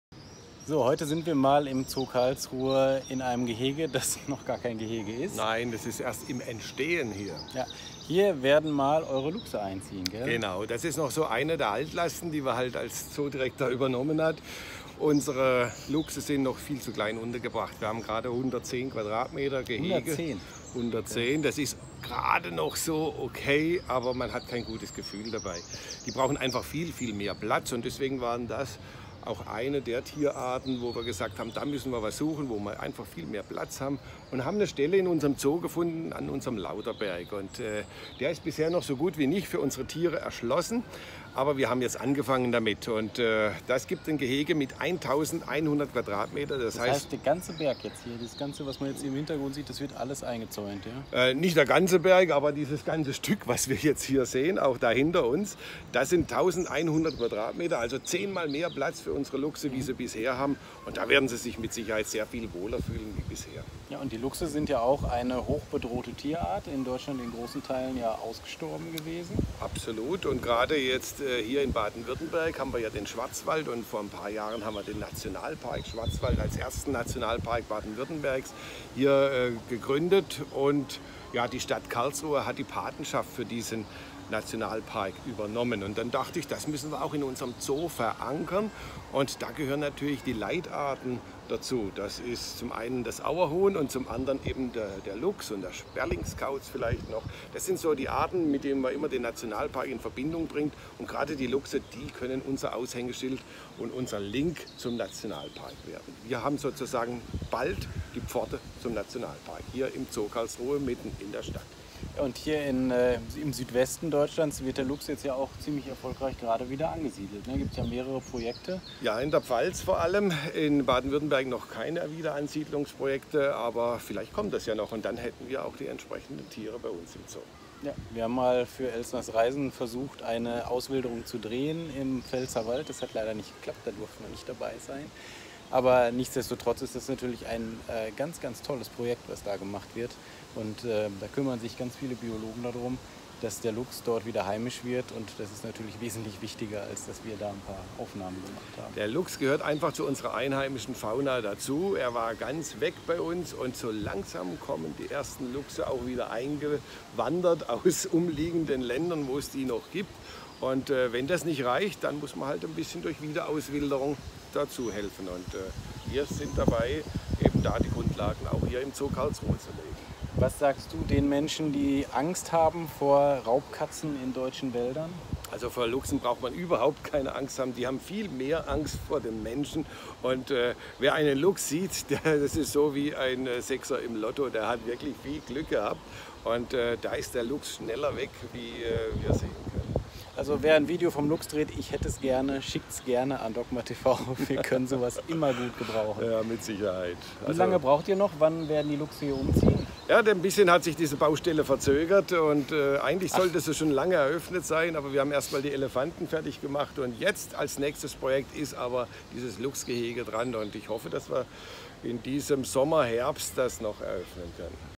Die Luchse im Karlsruher Zoo
Dieses Mal: Die Luchse bekommen ein neues Gehege... Einige von Euch haben nach den "alten" Filmen (2019) aus dem Zoo Karlsruhe mit Dr. Matthias Reinschmidt gefragt. Ab heute lade ich die alle hier noch mal hoch.